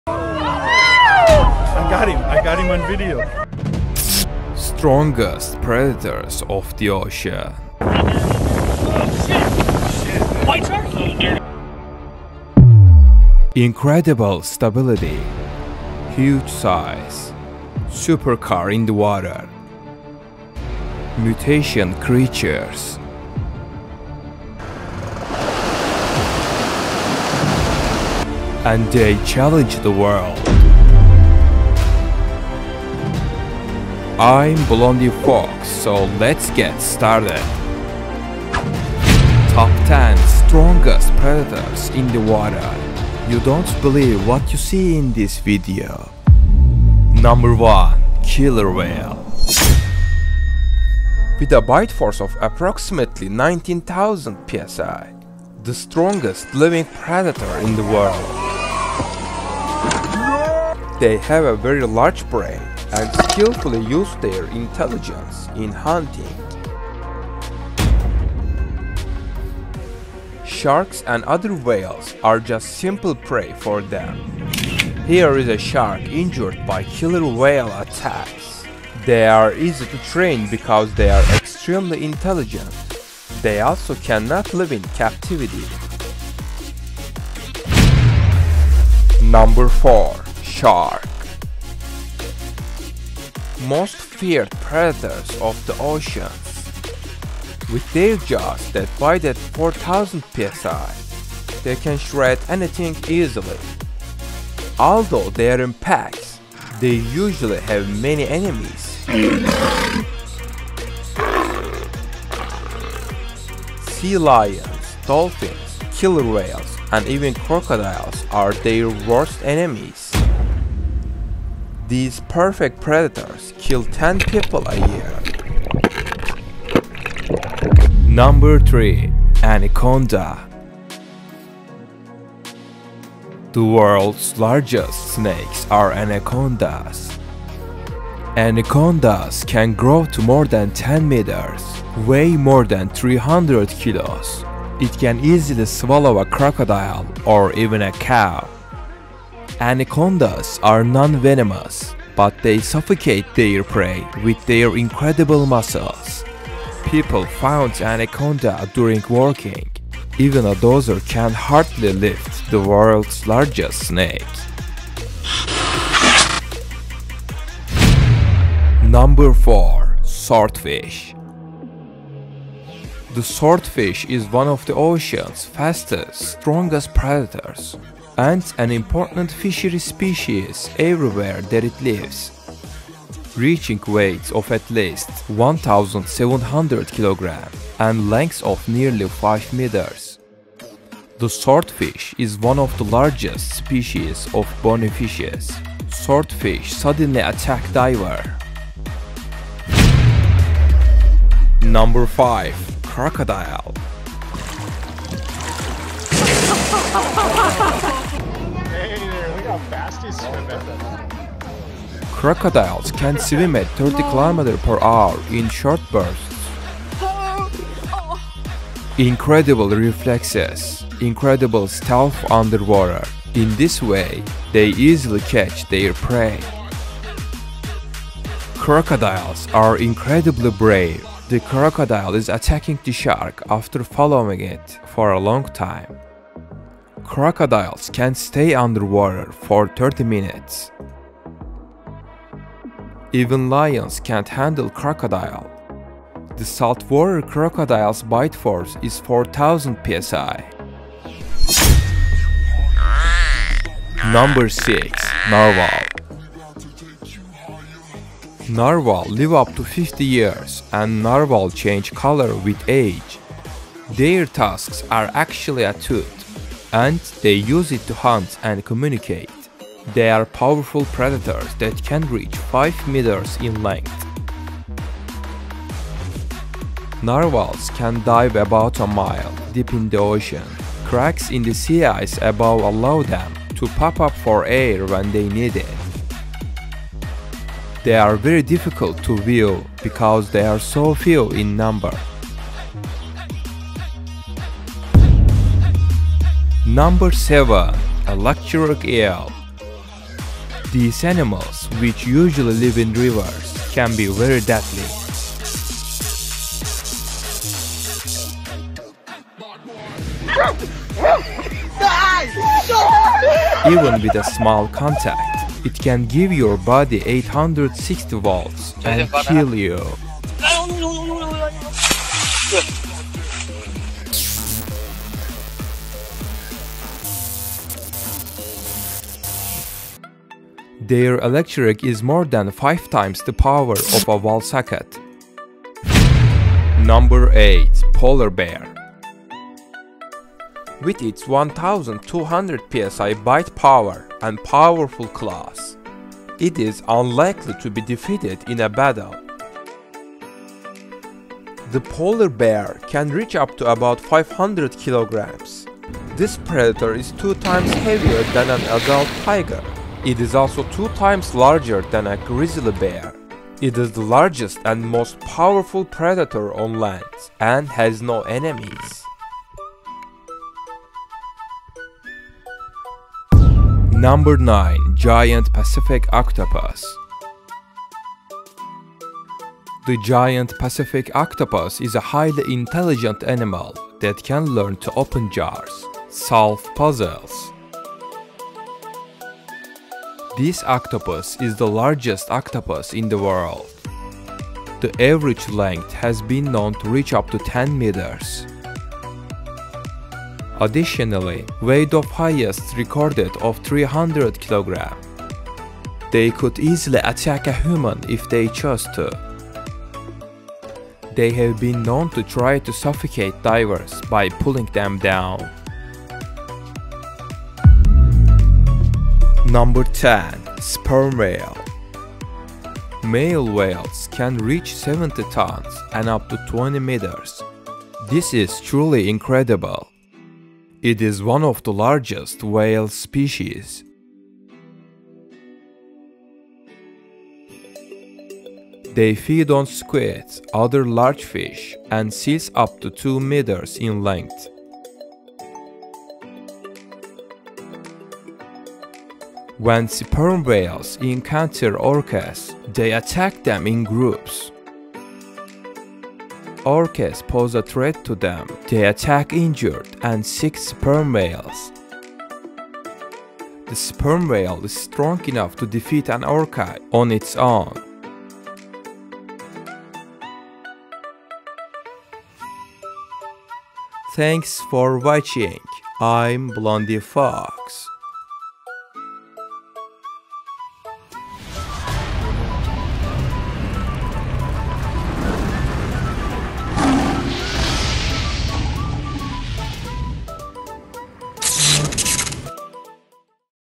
Top 10 Most Dangerous Sea Animals In The World! - Killers of Orcas!
Strongest predators of the ocean! Incredible stability, huge size, mutation creatures and they challenge the world! I'm Blondi Foks so let's get started! You won't believe what you see in this video! There were very dangerous animals in the oceans as well as on land. These animals shock us with their sizes. In this video, I listed the 10 most powerful marine animals and explained them to you in their nature along with their characteristics. I gave educational information about bite strength and weight. I hope you like my video.

Keep watching!

👉 You can activate the subtitles (All languages are active!)

Strongest predators of the ocean!
incredible stability
Huge size
Supercar in the water!
mutation creatures
and they challenge the world!
I'm Blondi Foks so let's get started!
Top 10 strongest predators in the water!
You won't believe what you see in this video

Number 1 Killer Whale
With a bite force of approximately 19,000 psi, the strongest living predator in the world.
They have a very large brain and skillfully use their intelligence in hunting.
Sharks and other whales are just simple prey for them.
here is a shark injured by killer whale attacks
They are easy to train because they are extremely intelligent.
they also cannot live in captivity

Number 2 Shark
Most feared predator of the oceans..
With their jaws that bite at 4000 psi, they can shred anything easily.
Although they are in packs, they usually have many enemies.
Sea lions, dolphins, killer whales and even crocodiles are their worst enemies.
These perfect predators kill 10 people a year

Number 3 Anaconda
The world's largest snakes are anacondas. Anacondas can grow to more than 10 meters, weigh more than 300 kilos.
It can easily swallow a crocodile or even a cow.
Anacondas are non-venomous, but they suffocate their prey with their incredible muscles.
People found anaconda during working. Even a dozer can hardly lift the world's largest snake.

Number 4 Swordfish
The swordfish is one of the ocean's fastest, strongest predators and an important fishery species everywhere that it lives.
Reaching weights of at least 1700 kg and lengths of nearly 5m, the swordfish is one of the largest species of bony fishes.
Swordfish suddenly attack diver.

Number 5 Crocodile
Crocodiles can swim at 30 km/h in short bursts.Incredible reflexes, incredible stealth underwater. In this way, they easily catch their prey.
Crocodiles are incredibly brave. The crocodile is attacking the shark after following it for a long time.
Crocodiles can stay underwater for 30 minutes.
The saltwater crocodile's bite force is 4000 psi.

Number 6 Narwhal
Narwhal live up to 50 years and narwhal change colour with age.
Their tusks are actually a tooth and they use it to hunt and communicate.
They are powerful predators that can reach 5 meters in length.
Narwhals can dive about a mile deep in the ocean. Cracks in the sea ice above allow them to pop up for air when they need it.
They are very difficult to view because they are so few in number.

Number 7 Electric Eel
These animals, which usually live in rivers, can be very deadly.
Even with a small contact, it can give your body 860 volts and kill you.
Their electric is more than five times the power of a  wall socket!

PREDATOR
Predators are wild animals that hunt, or prey on, other animals. All animals need food to live. Predator animals need the flesh of the animals that they kill to survive. Orcas, sharks, crocodiles, anacondas are all predators. Predators are carnivores, which means their diet consists of meat. Some predators, such as coyotes and bears, are also scavengers, meaning they will eat the carcasses of animals that they didn't hunt themselves.

❗I may have used small parts of your video. I tried to give credit to all videos.
- Blondi Foks.